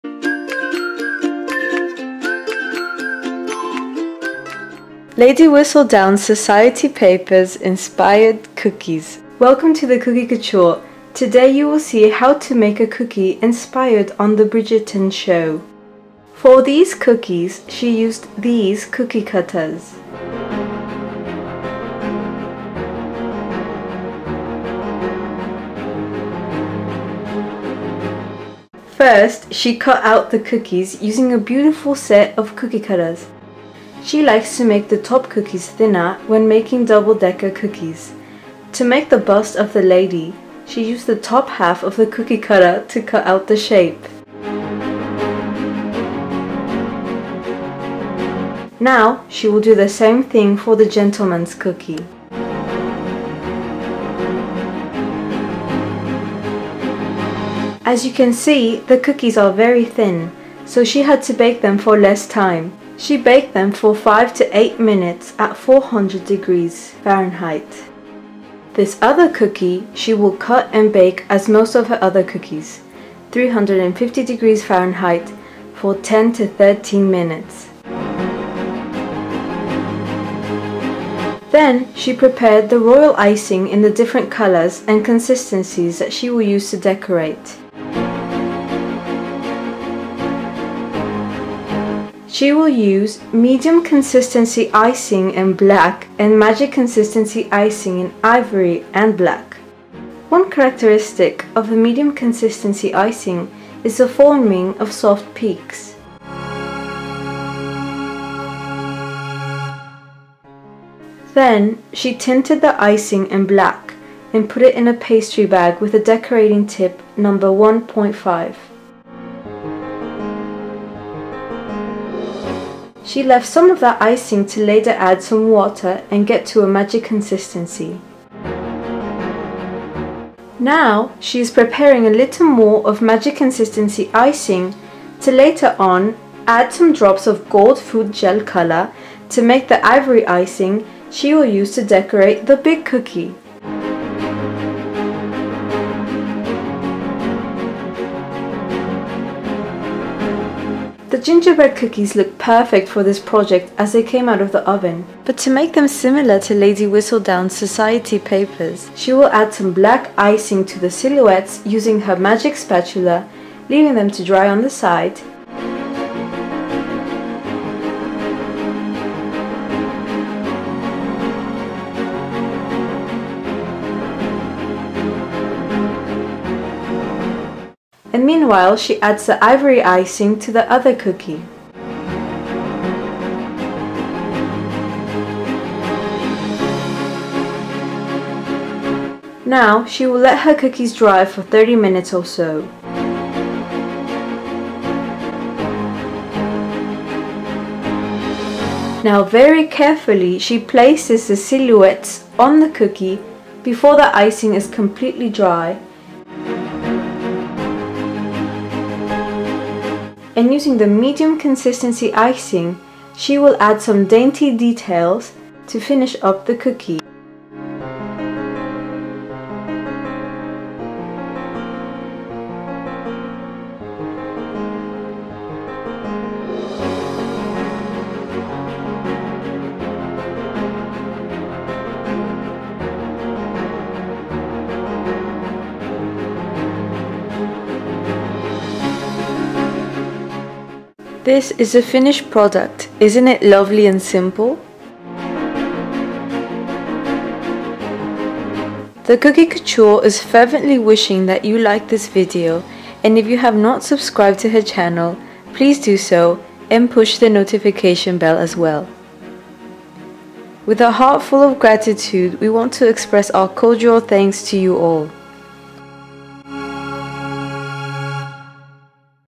Lady Wistledown's inspired cookie
In this video you will see how to decorate cookies inspired in Lady Wistledown's society papers from the show Bridgerton, this cookie is a very simple cookie to make that can be a perfect DIY Valentine's Day gift.

Amazon
Kitchen Aid